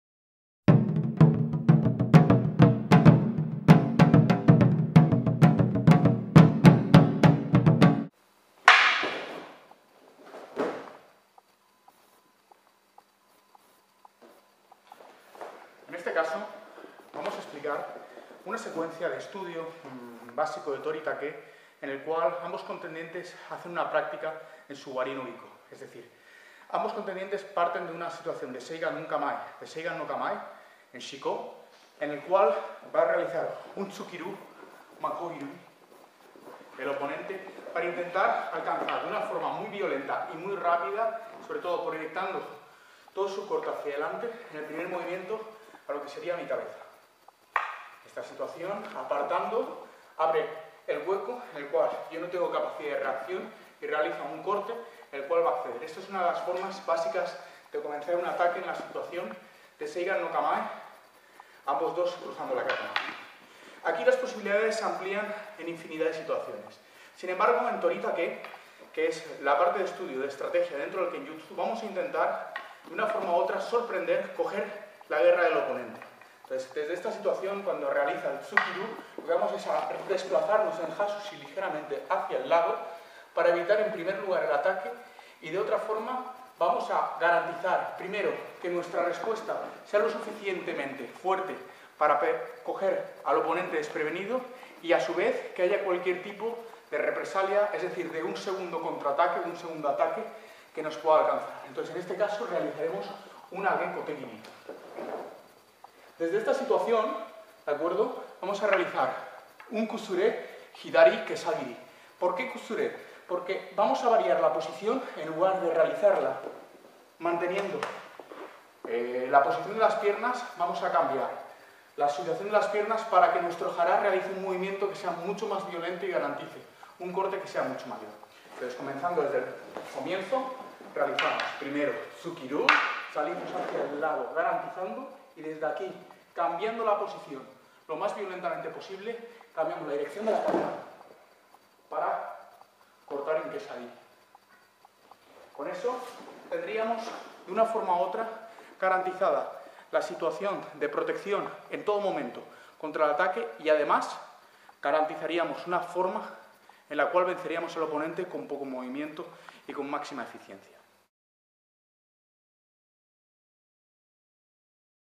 Ogawa Ryu Kaishaku Serie - Kenjutsu Toritake II
Luis Nogueira explains kenjutsu - Toritake